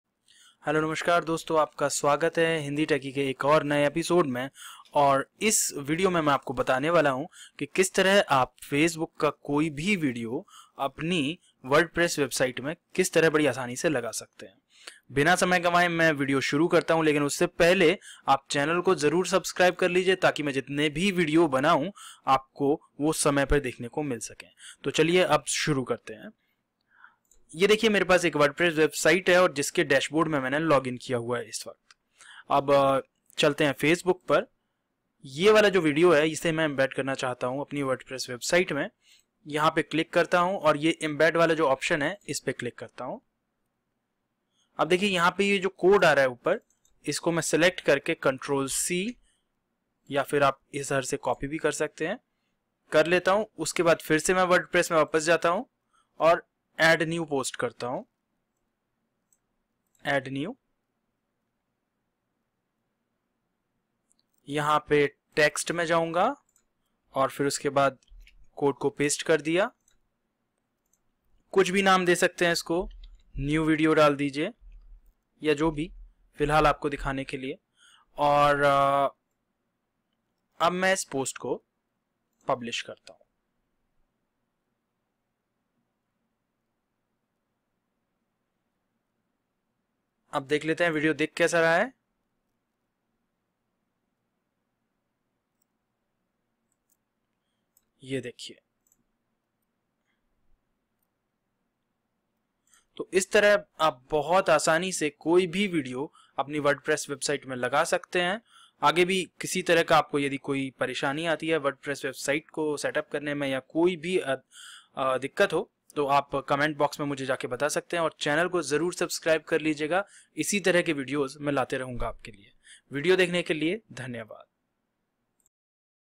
How to embed Facebook video in wordpress site without plugin in Hindi
Hi Friends, iss video me aap janenge ki kis tarah aap apni wordpress website me facebook video laga sakte hain bina kisi plugin ke, How to embed Facebook video in wordpress site without plugin in Hindi